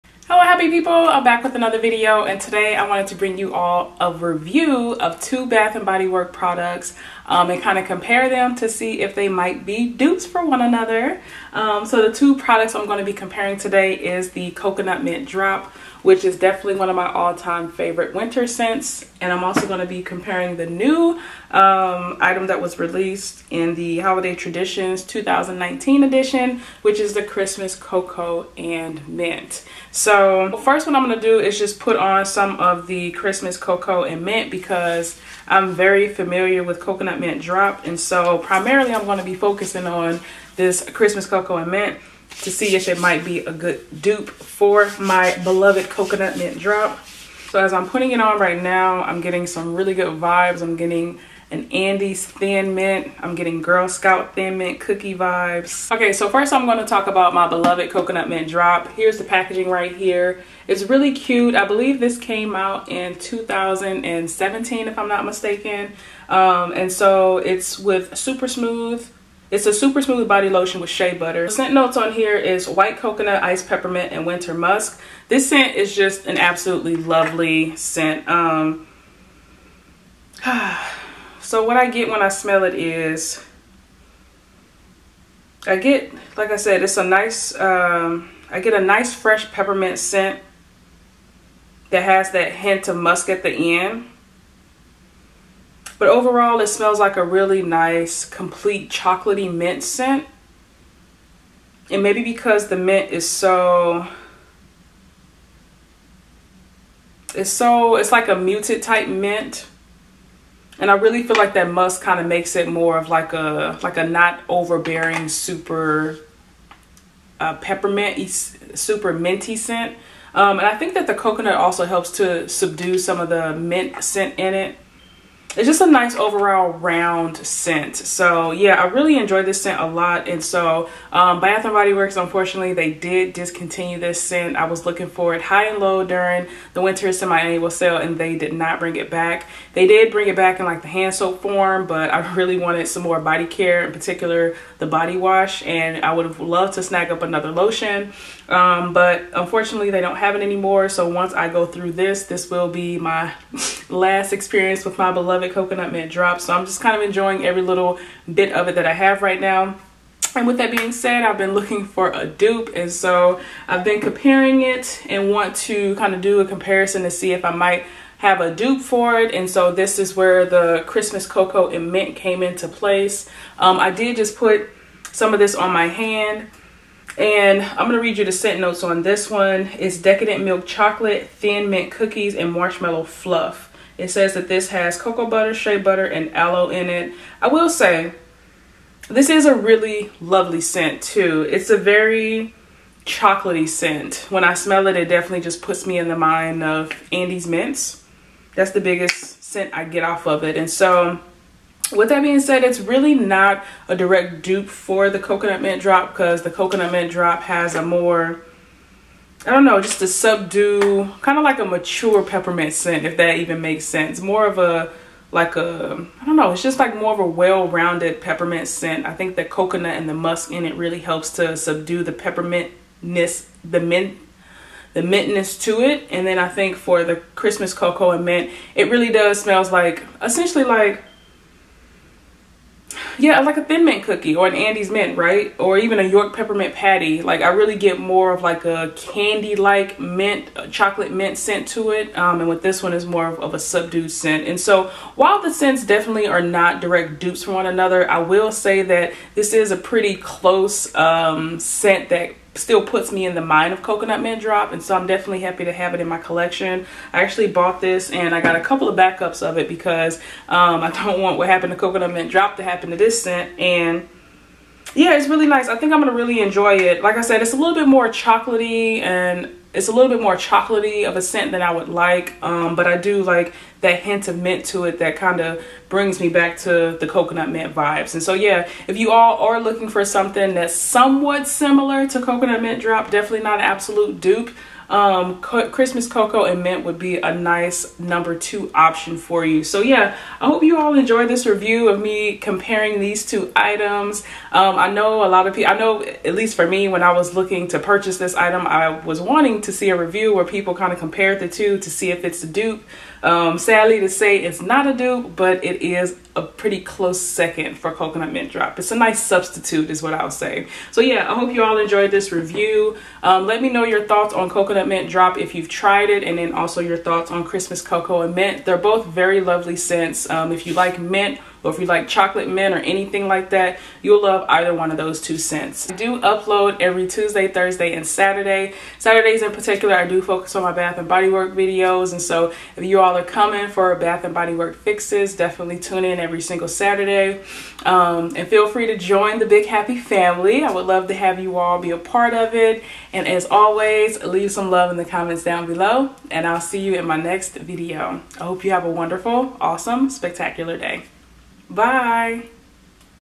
Is it a Dupe? | Coconut Mint Drop vs Christmas Cocoa and Mint Review | Bath and Body Works | BBW
Hello happy people, in this video I give my honest thoughts and compare the Bath and Body Works Coconut Mint drop vs the Christmas Cocoa and Mint Collections. Please leave your thoughts on this scent below. Enjoy.

Coconut Mint Drop Lotion

Christmas Cocoa and Mint